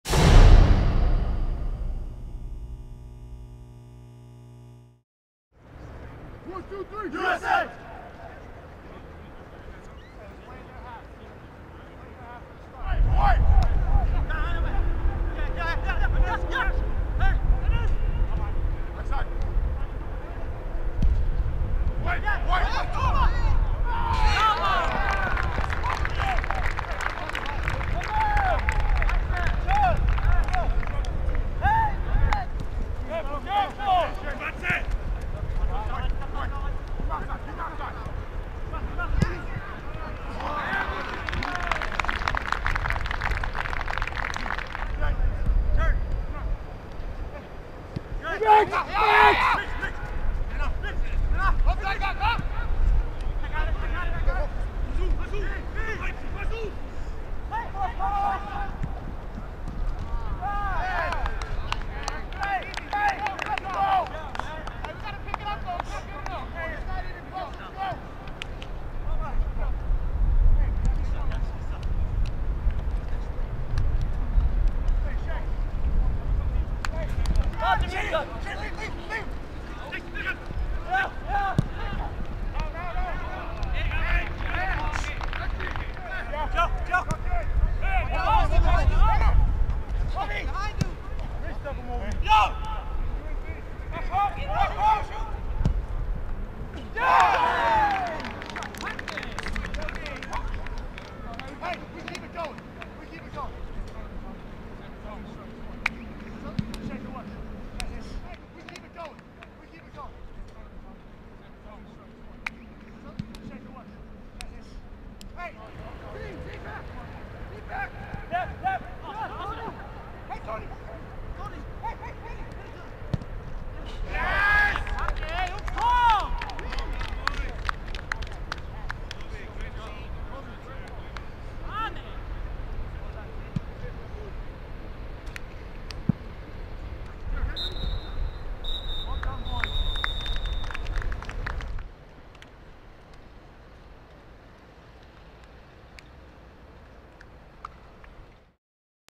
U-23 MNT vs. VfL Bochum: Highlights - Nov. 11, 2011
The U.S. Under-23 Men took on VfL Bochum in a scrimmage in Germany during their preparations for 2011 Olympic qualifying. Terrence Boyd, Tony Taylor and Dilly Duka scored the goals in a 3-1 win for the USA.